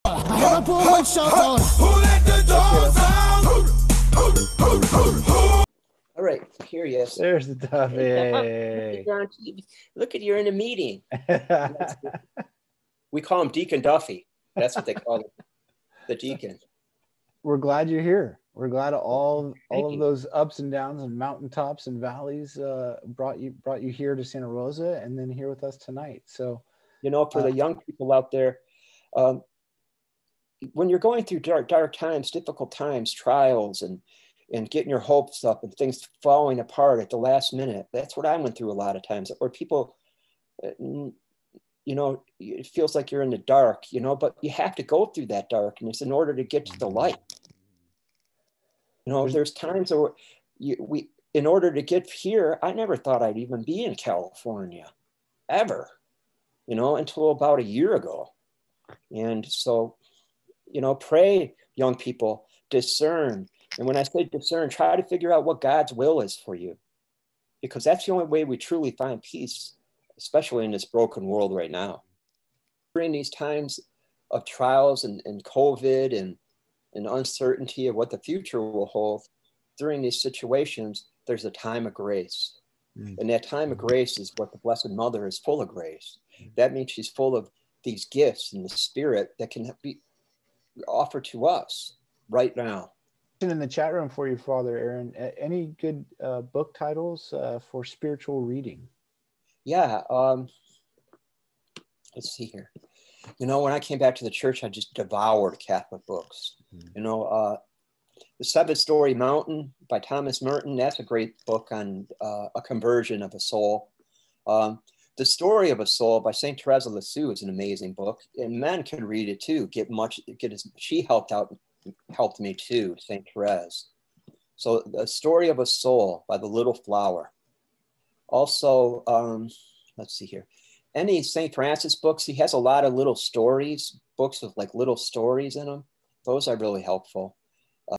SR Diocese AskaPriest #3 Fr Aaron
Our Diocesan Series "Ask a Priest" takes place 8pm Wednesdays from September thru Thxgiving. We spend an hour with one of our Diocesan Priests and begin with Prayer & Reflections on the Week's Scriptures and then open discussion and conversations.